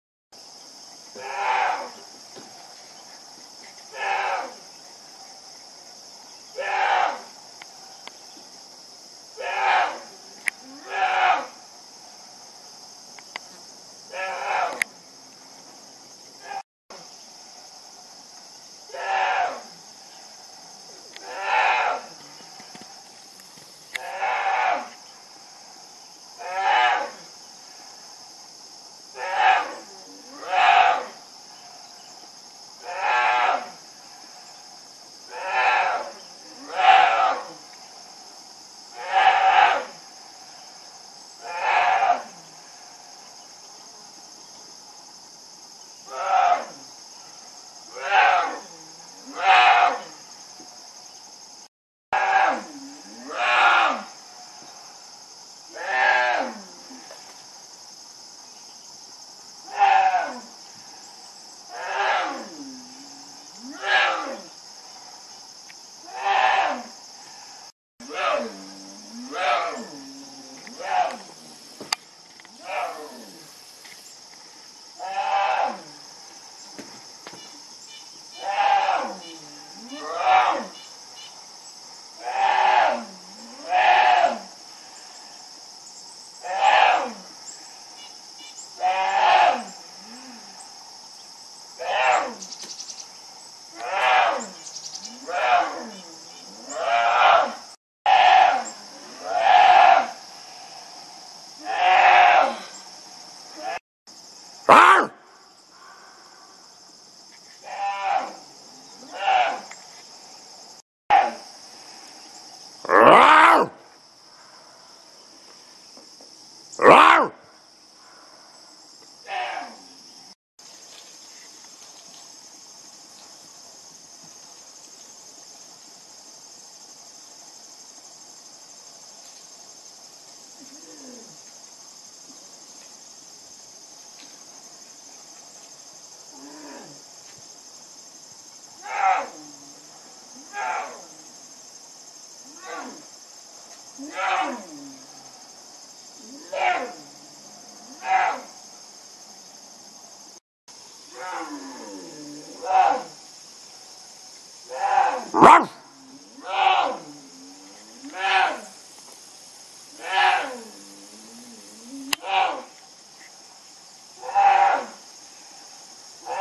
2015-09-04 Agitated Adolescent Bear at Grace Lake
We spotted an adolescent bear swimming quickly across Grace Lake and then hanging out around the cottage for at least 45 minutes bellowing for its mother (we assume). It bellowed for a half hour from the ground and from 40 ft up a tree until Mom arrived and rescued her idiot son.